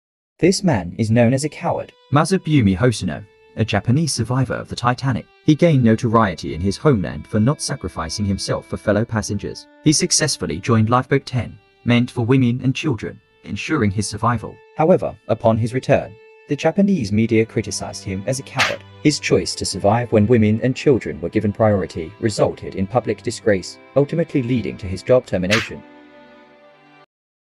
Most Coward Passenger on Titanic 🚢
Masabumi Hosono, a Japanese survivor of the Titanic, is widely criticized in his homeland for not sacrificing himself for fellow passengers. Managing to secure a place on lifeboat 10, which was intended for women and children, ensured his survival. However, upon his return, the Japanese media branded him a coward, igniting public shame and resulting in his job loss. Hosono's choice to prioritize his own survival over that of women and children stirred significant controversy and condemnation within his society.